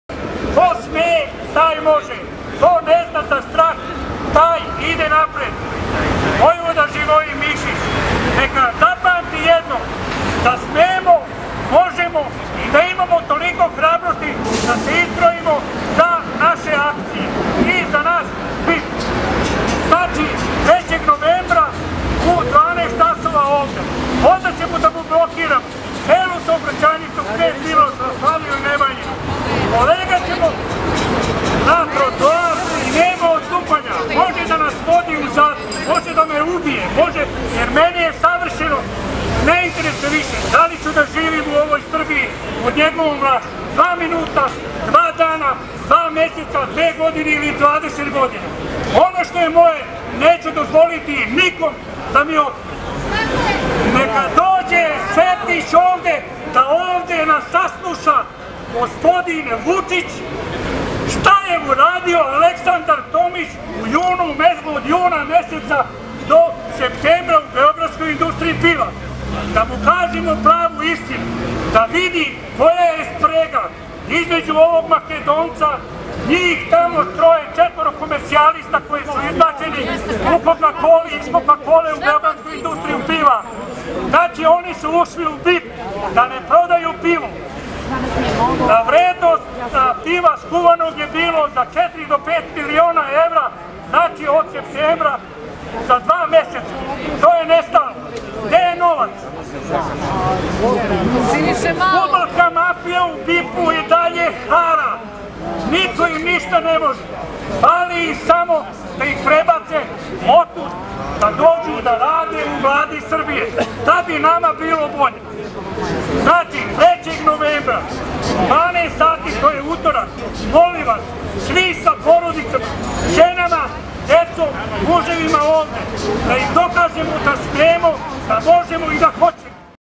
Protest against bancrupcy of BIP
Employees and shareholders gather on a protest against the sale of Belgrade's Beer Industry - BIP in front of the Government building.